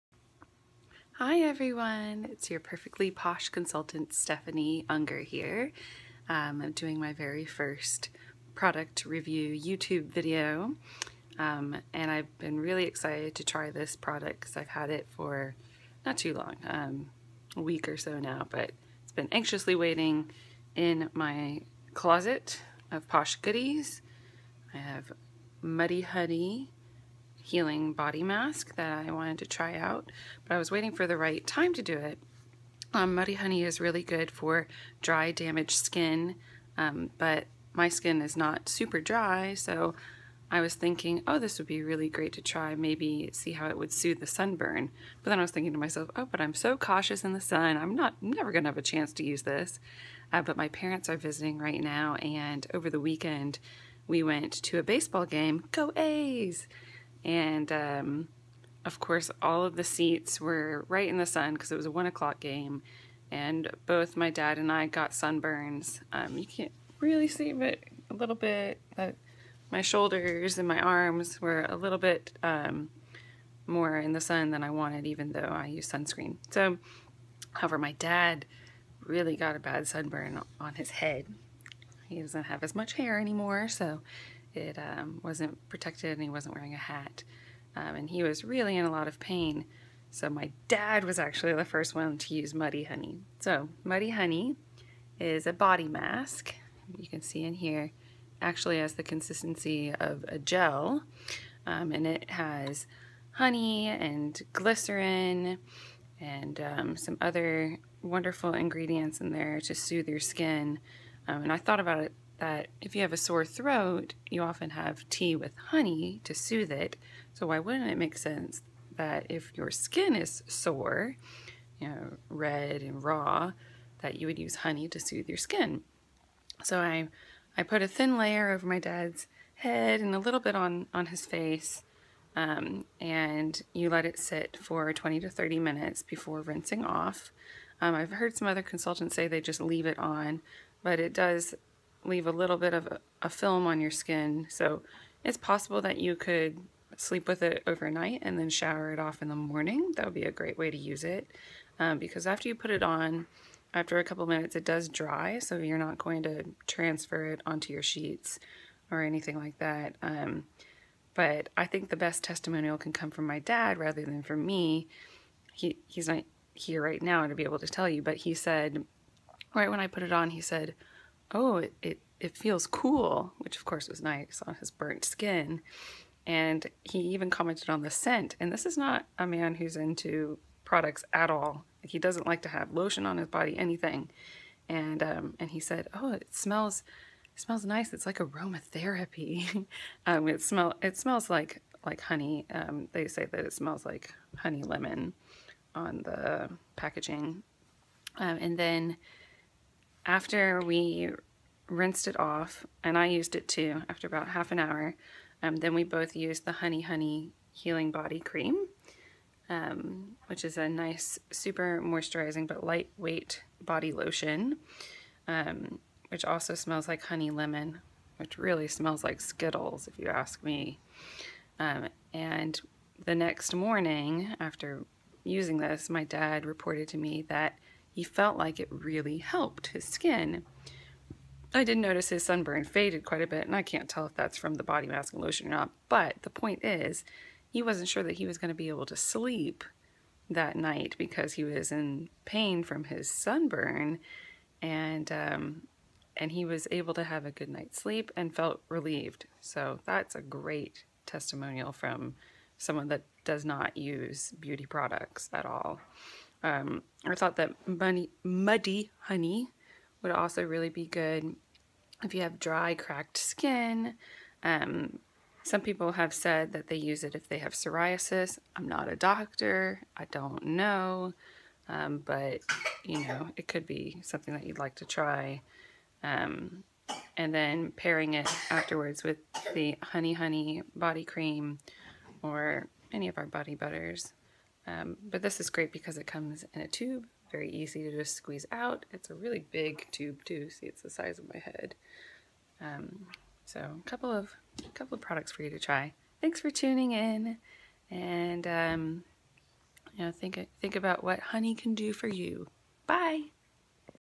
Perfectly Posh Muddy Honey and Honey Honey product reviews
Perfectly Posh product review Muddy Honey body mask and Honey Honey body creme